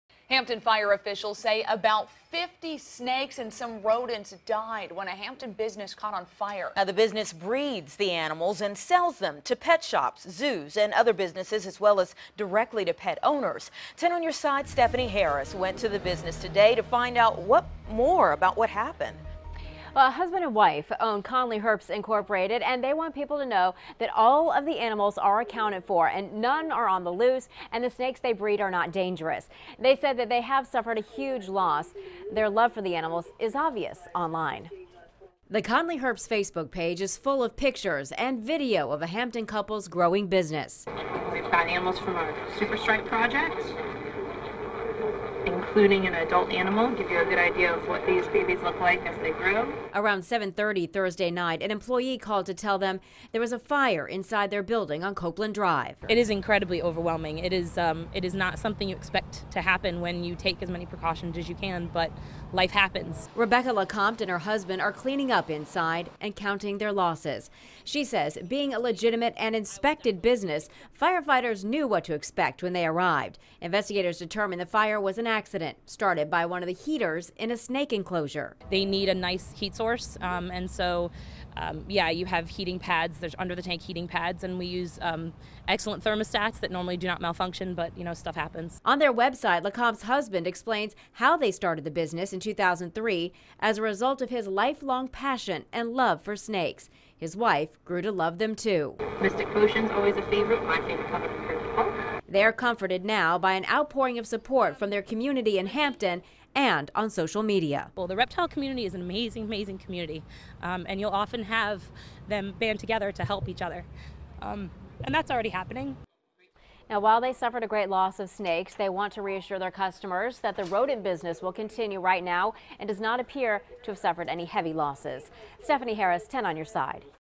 About 50 snakes, significant amount of rodents die in Hampton fire
The fire was sparked by electrical heaters used in the snake enclosures.